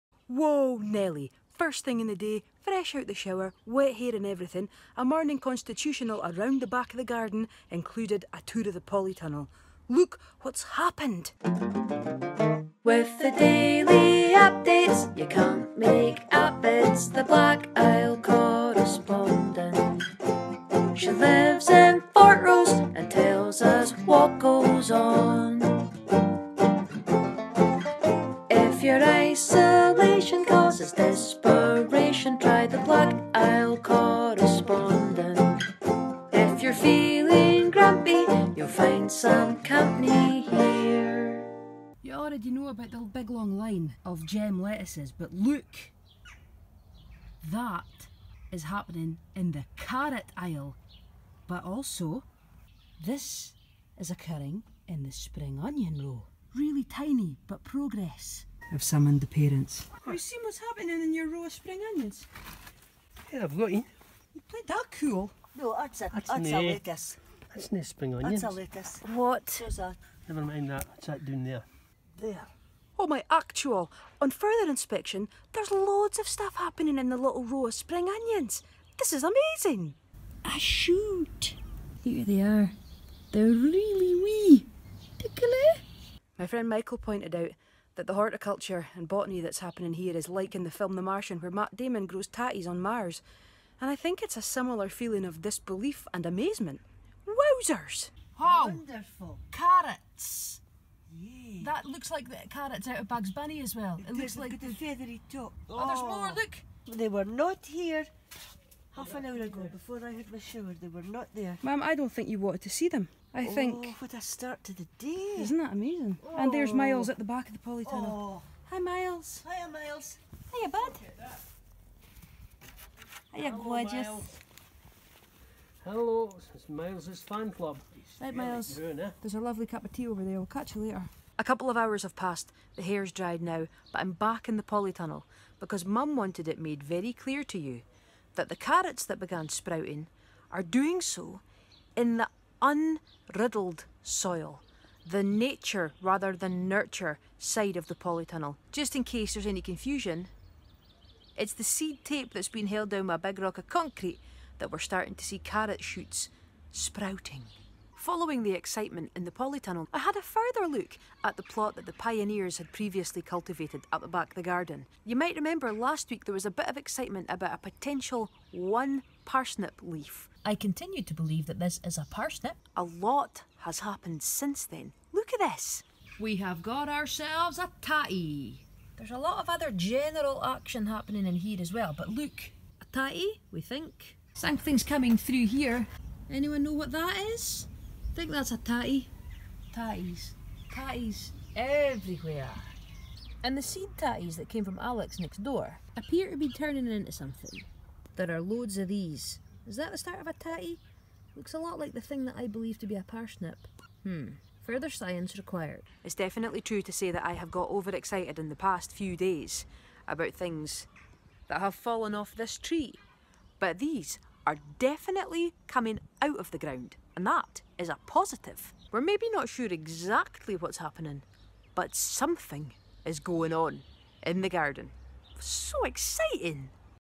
BIC 57 - Polytunnel developments
Black Isle Correspondent episode 57: Wasn't intending another update from the growth tank so too, but this was an exciting day!